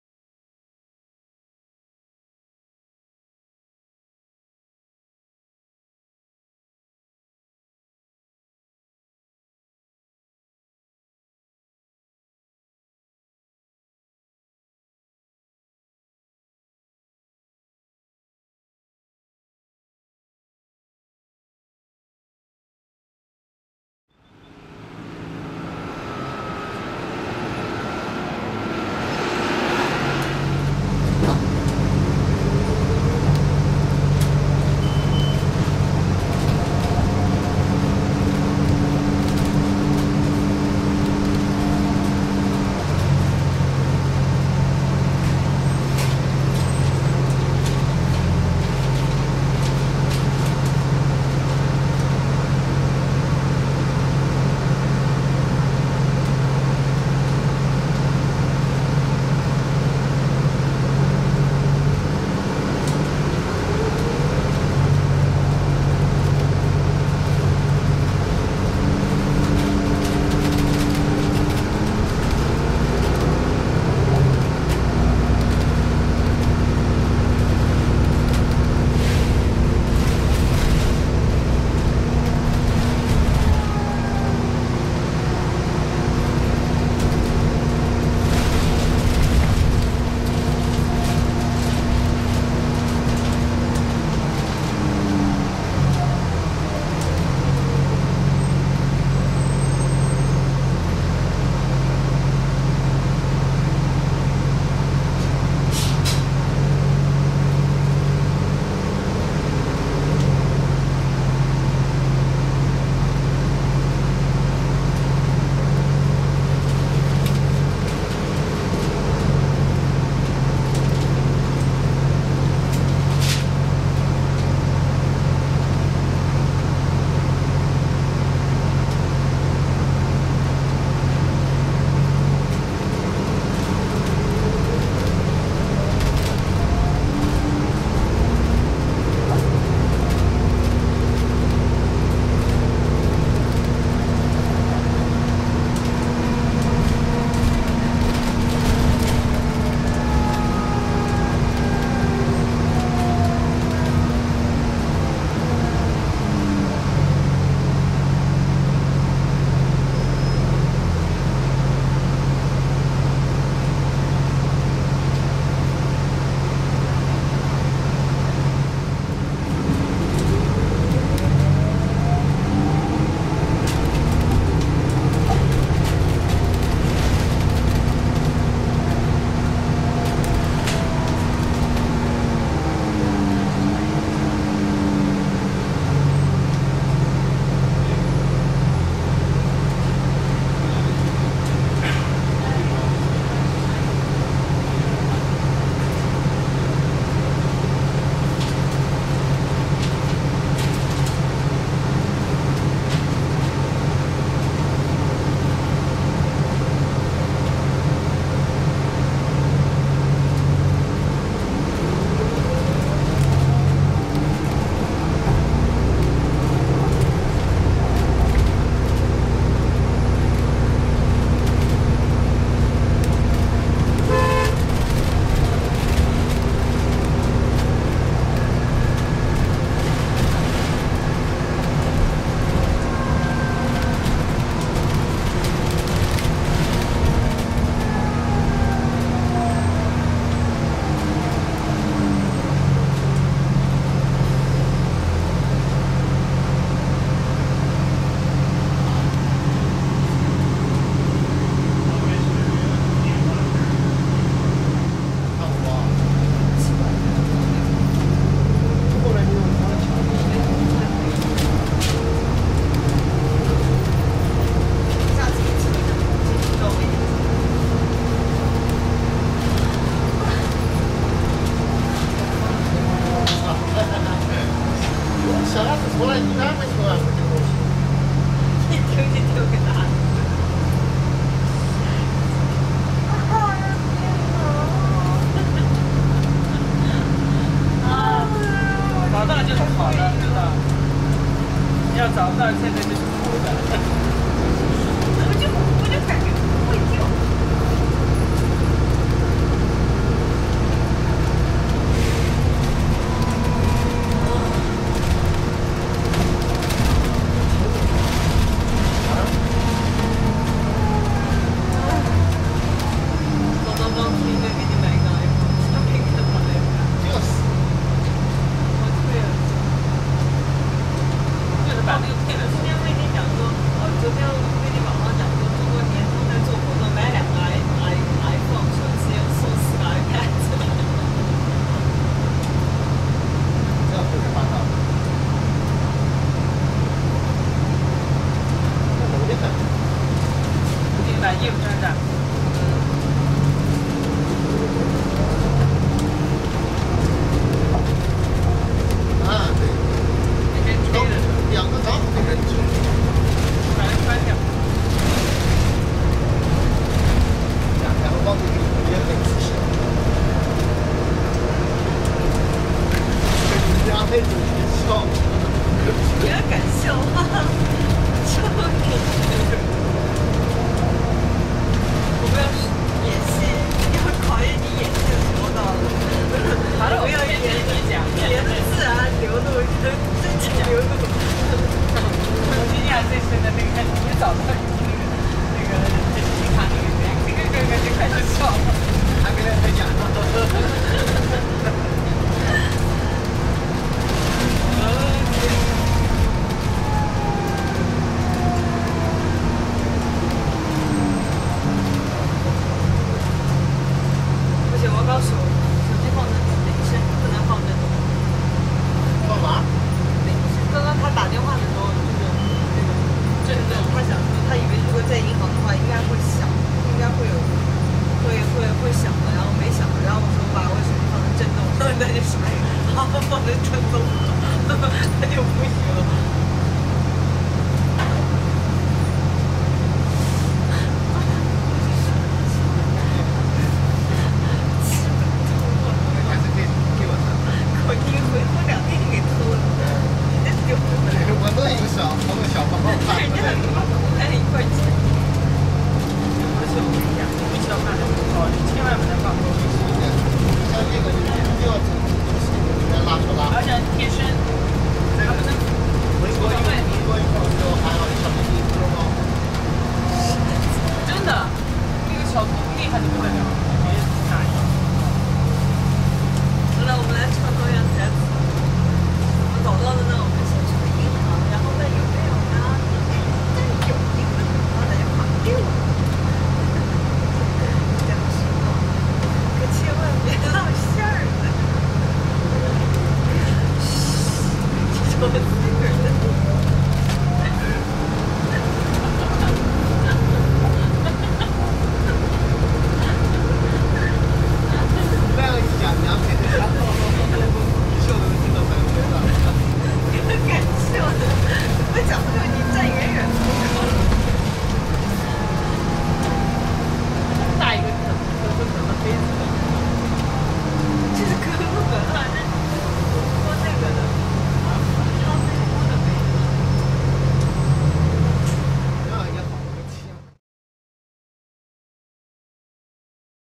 BC Transit Victoria 2008 Alexander Dennis Enviro 500 Hybrid - 9527 (Part 1)
Alexander Dennis was introduced the diesel-electric hybrid version of Enviro 500 double decker to BC Transit in 2008, it was displayed at the APTA Expo convention show in San Diago, CA before delivery to BC Transit~~

BC Transit was received this bus in 2009 with the 6 months trial, but it was sent back to Alexander Dennis for modification after trial, and came back to BC Transit in 2010 and displayed at the Trans-Expo convention show in Vancouver, BC~~

For the powertrain, this bus equipped Cummins ISL-330 EPA2007 engine and Allison Ep50 parallel hybrid drive, air-con system provided by Thermo-King~~

Let's go to the part 1 joyride of the Alexander Dennis E500H - 9527 during on route 70x toward Swartz Bay, the part 1 journey was taken at the urban areas in Victoria, BC~~